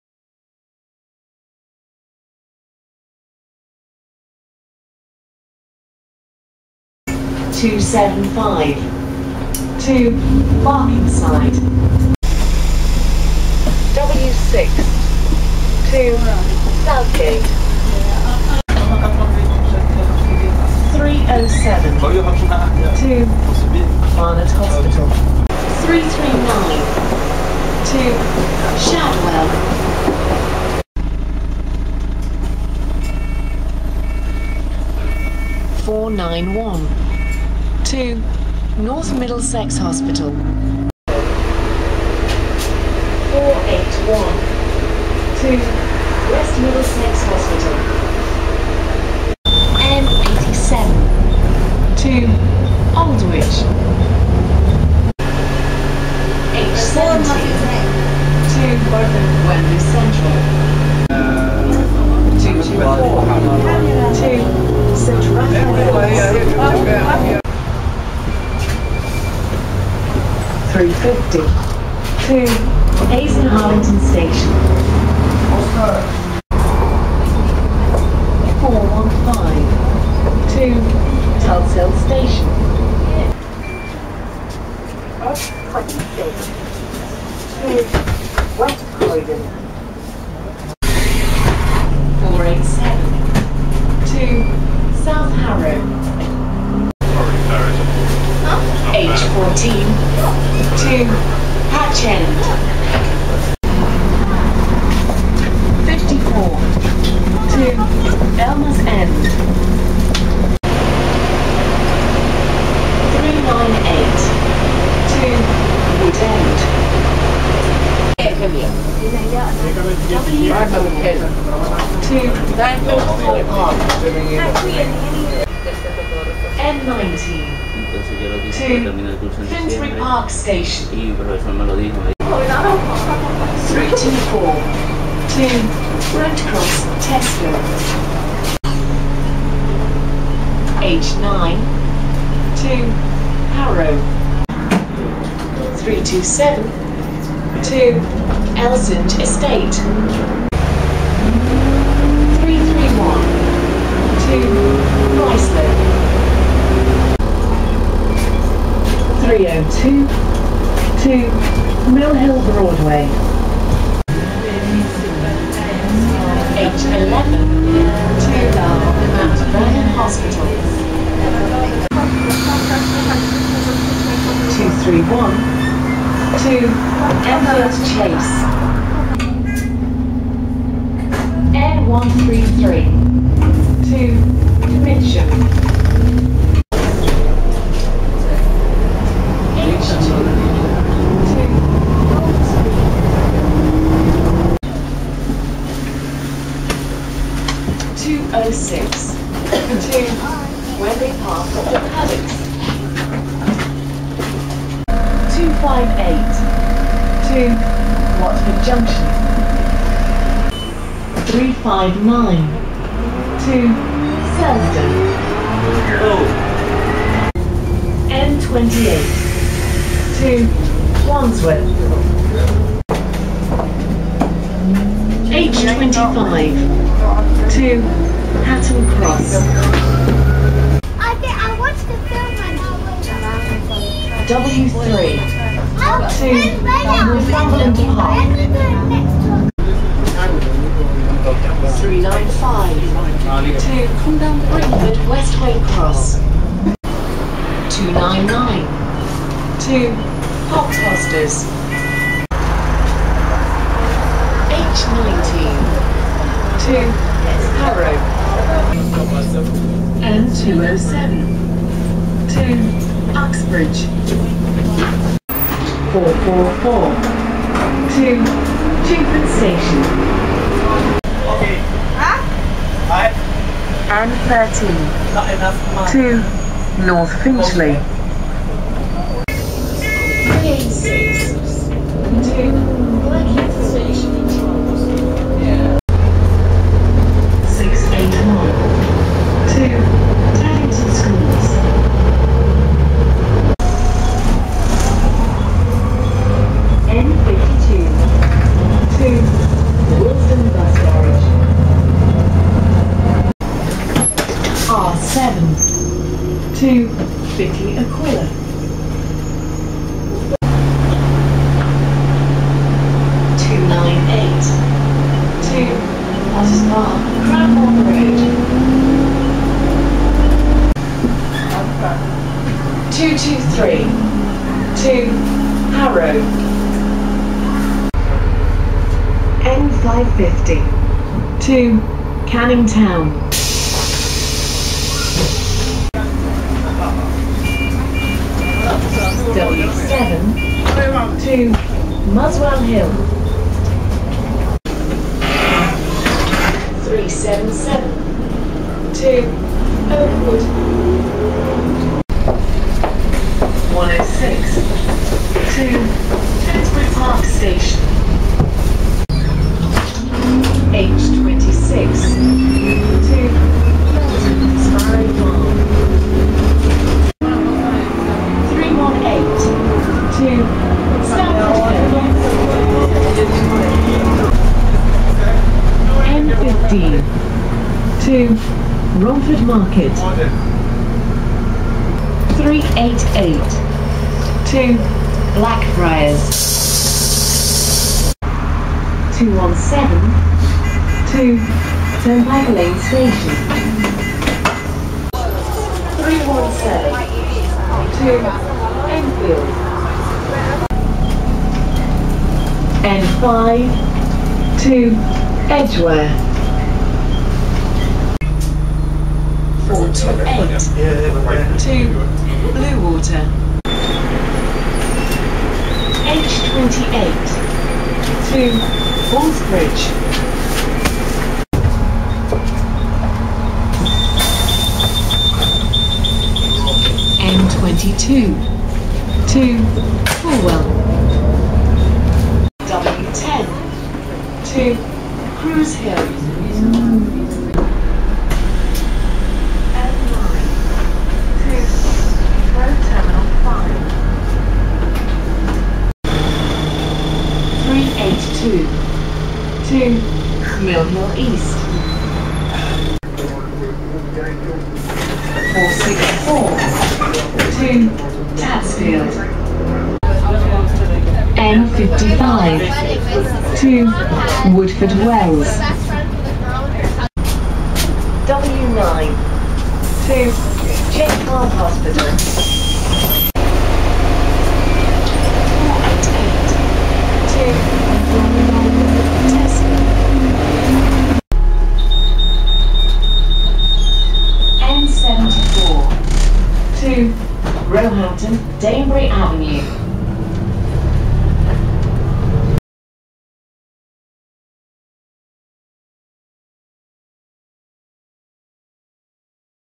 iBus: Route Changes, iBus Updates and Never Done in Past Videos 2
Video Insight

Featuring routes from bus operators Abellio London, Arriva, CT Plus, First, Go Ahead London, London United, Quality Line, Metrobus, Metroline, Quality Line, Stagecoach, Sullivan Buses and Transdev.

Route Changes:
0:19 Route 307 Withdrawn Between Barnet, Arkley Hotel and Wood Street, Diverted to Barnet Hospital on 2nd June 2012.
0:25 Route 339 Extended from Bow, Fish Island to Stratford City on 13th September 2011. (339 to Shadwell)
1:01 Route 224 Withdrawn Between Willesden Junction and Brent Park, Extended to from Brent Park to St. Raphael's on 15th October 2011.
1:51 Route 398 Withdrawn Between Greenford, Westway Cross and Northolt Park, Extended from Northolt Park to Wood End on 5th September 2009.
2:13 Route 324 New Route Introduced on 23rd October 2010.
3:15 Route 206 Withdrawn Between St. Raphael's and Brent Park, Extended from Brent Park - Wembley Park, The Paddocks on 15th October 2011.
4:00 Route 395 New Route Introduced on 5th September 2009.
5:04 Route R7 Extended from Bickley to Bickley, Aqulia on 27 March 2010.
6:15 Route N15 Withdrawn Between Paddington Basin and Regent Street on 28th August 2010. (N15 to Romford Market)
6:22 Route 388 Withdrawn Between Embankment and Blackfriars on 23rd March 2012.
7:54 Route 488 Withdrawn Between Twelvetrees Crescent and Bromley-By-Bow Tesco on 9th April 2010.

iBus Updates:
0:07 275 to Barkingside, Tesco - 275 to Barkingside
1:17 415 to Tulse Hill - 415 to Tulse Hill Station
1:24 X26 to Croydon Town Centre - X26 to West Croydon
1:44 54 to Elmers End Green - 54 to Elmers End
4:39 386 to Blackheath - 386 to Blackheath Village
5:53 106 to Finsbury Park - 106 to Finsbury Park Station
6:54 H28 to Bull's Bridge, Tesco - H28 to Bull's Bridge

Routes Never Done:
0:12 W6 to Southgate
0:25 339 to Shadwell
0:31 491 to North Middlesex Hospital
0:41 481 to West Middlesex Hospital
0:49 N87 to Aldwych
0:55 H17 to Wembley Central
1:01 224 to St. Raphael's
1:08 350 to Hayes & Harlington Station
1:31 487 to South Harrow
1:37 H14 to Hatch End
1:51 398 to Wood End
1:58 W4 to Oakthorpe Park
2:06 N19 to Finsbury Park Station
2:13 324 to Brent Cross, Tesco
2:19 H9 to Harrow
2:25 327 to Elsinge Estate
2:31 331 to Ruislip
2:38 302 to Mill Hill Broadway
2:44 H11 to Mount Vernon Hospital
2:52 231 to Enfield Chase
2:59 N133 to Mitcham
3:07 H2 to Golders Green
3:15 206 to Wembley Park, The Paddocks
3:24 258 to Watford Junction
3:31 359 to Selsdon
3:37 N28 to Wandsworth
3:43 H25 to Hatton Cross
3:51 W3 to Northumberland Park
4:00 395 to Greenford, Westway Cross
4:07 299 to Cockfosters
4:12 H19 to Harrow
4:19 N207 to Uxbridge
4:25 444 to Chingford Station
4:31 N13 to North Finchley
4:46 681 to Teddington Schools (Bonus)
4:54 N52 to Willesden, Bus Garage
5:11 298 to Potters Bar, Cranborne Road
5:21 223 to Harrow
5:28 N550 to Canning Town
5:36 W7 to Muswell Hill
5:45 377 to Oakwood
5:59 H26 to Feltham, Sparrow Farm
6:07 318 to Stamford Hill
6:14 N15 to Romford Market
6:28 217 to Turnpike Lane Station
6:35 317 to Enfield
6:41 N5 to Edgware
6:48 428 to Bluewater
7:02 N22 to Fulwell
7:12 W10 to Crews Hill
7:18 N9 to Heathrow Terminal 5
7:26 382 to Mill Hill East
7:33 464 to Tatsfield
7:40 N55 to Woodford Wells
7:47 W9 to Chase Farm Hospital
8:02 N74 to Roehampton, Danebury Avenue